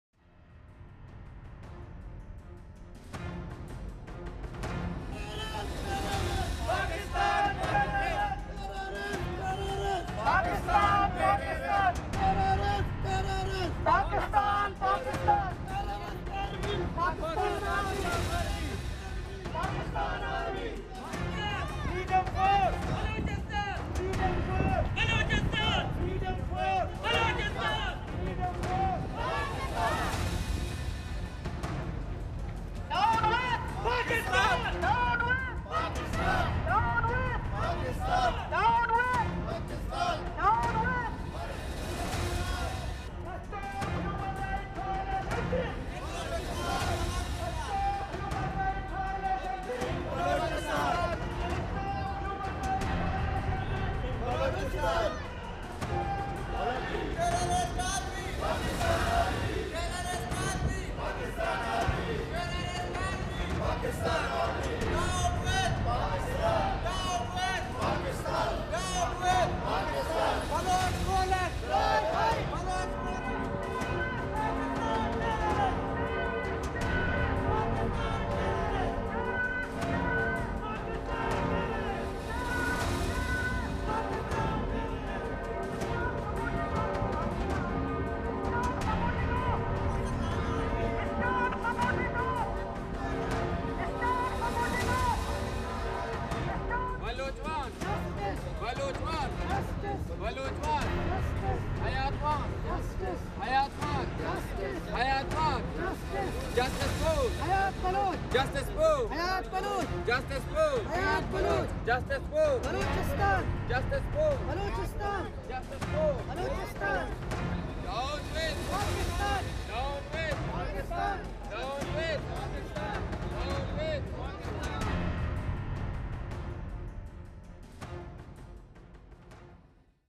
Free Balochistan Movement protests in Germany against killing of 25-yr-old student Hayat Baloch
Hamburg Germany, September 7 (ANI): The Free Balochistan Movement organized a demonstration and a protest rally against the murder of Hayat Mirza Baloch in the Hamburg city of Germany. According to details, the demonstration was organized on Saturday at 5 PM at Hachmannplatz Hamburg where the protesters marched from different streets of the city to Gaensemarkt. Protesters were holding banners and placards inscribed with different slogans demanding justice for Hayat Baloch. Protesters also distributed thousands of pamphlets and leaflets during the protest. People from the German civil society and representatives from different political parties also participated in the protest to show their solidarity with Baloch people and express their anger against the extra-judicial murder of Baloch students by the Pakistan Army. Stephen Brant, during his speech, said that Balochistan was divided into three parts by Britain during their colonial rule in the region, by drawing two arbitrary lines, which are at the moment being ruled by three different countries. Free Balochistan Movement activist Naveed Baloch said that 25-year-old Karachi University Student Hayat Mirza Baloch and the military and government is trying to protect his murderer. He also said that we demand the arrest of heads of FC and all those officials who were present at the time of Hayat's killing. Hayat Baloch was killed extra-judicially on August 13 by occupying Pakistani forces in front of his elderly parents shot eight (8) times in the Turbat region of Balochistan. Entire Balochistan erupted with protest after the brutal murder of the young Baloch student and demands for justice for Hayat Baloch continue to grow.
Newsweek South Asia, News Week South Asia, Newsweek South Asia channel